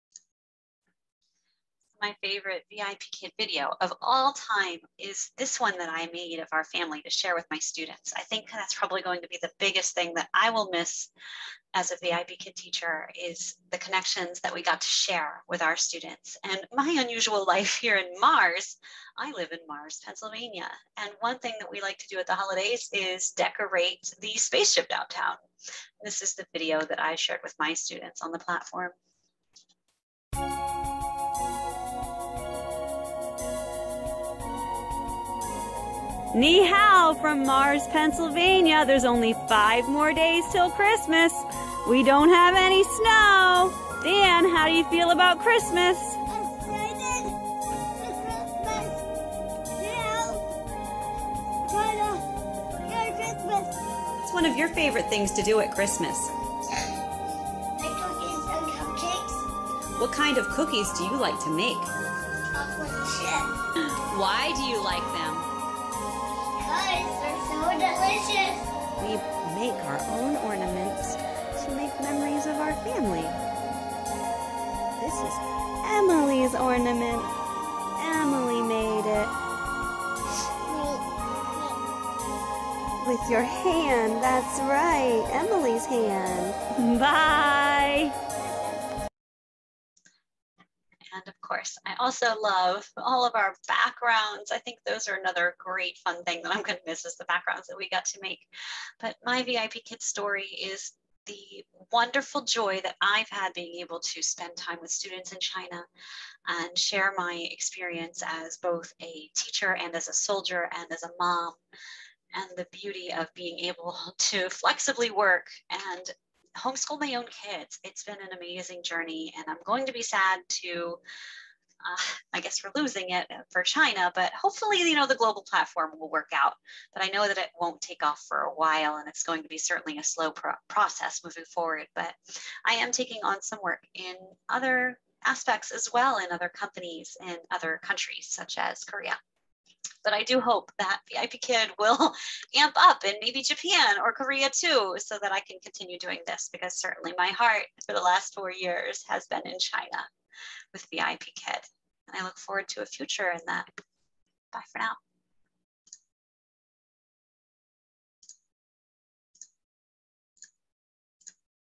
Merry Christmas Ni Hao from Mars PA
As we end the chapter on our lives in China, I wanted to capture some of the things I loved about working here. It's heartbreaking to say goodbye to Vipkid. I remain hopeful that it isn't really goodbye, but more of an until next time. As of now, I have only been invited to Book Nook which I'm not able to do. I have not yet been invited to the global program.